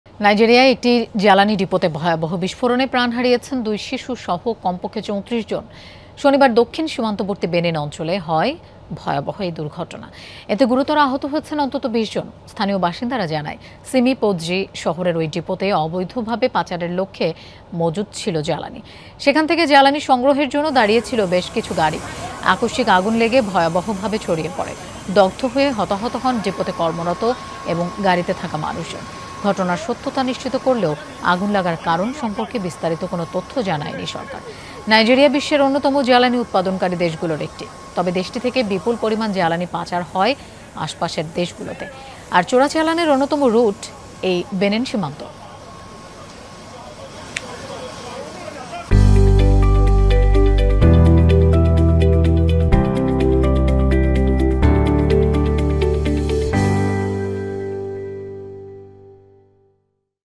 নাইজেরিয়ায় অবৈধ তেলের ডিপোয় ভয়াবহ বিস্ফোরণ; নিহত অন্তত ৩৪ | Nigeria Blast | Jamuna TV
নাইজেরিয়ায় একটি জ্বালানি ডিপোতে ভয়াবহ বিস্ফোরণে প্রাণ হারিয়েছেন দুই শিশুসহ কমপক্ষে ৩৪ জন। শনিবার, দক্ষিণ সীমান্তবর্তী বেনিন অঞ্চলে হয় ভয়াবহ এ দুর্ঘটনা।

Address:
Bangladesh.